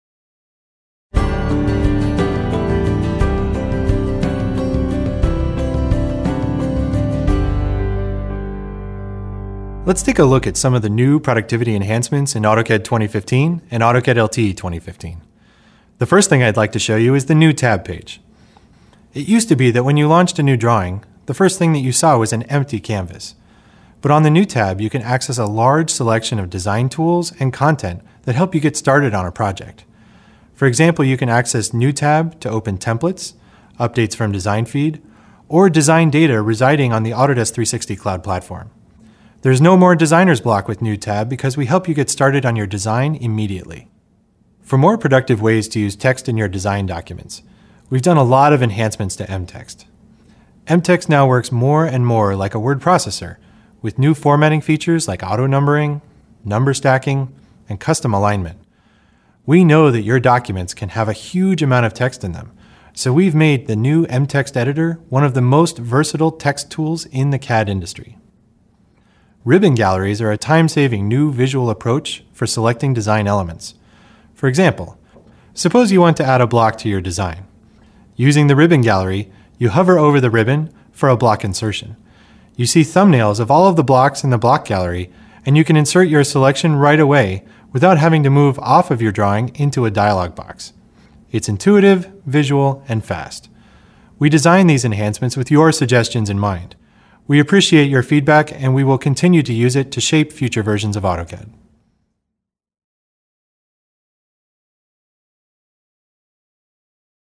AutoCAD LT 2016 - Ribbon Galleries
Save time and clicks by accessing drawing context directly from the ribbon. For example, to add a block to your design, hover over the ribbon for a block insertion. You can see thumbnails of all the blocks in the Block gallery and you can insert your selection straight away, without having to move off your drawing into a dialogue box. It's intuitive, visual and fast.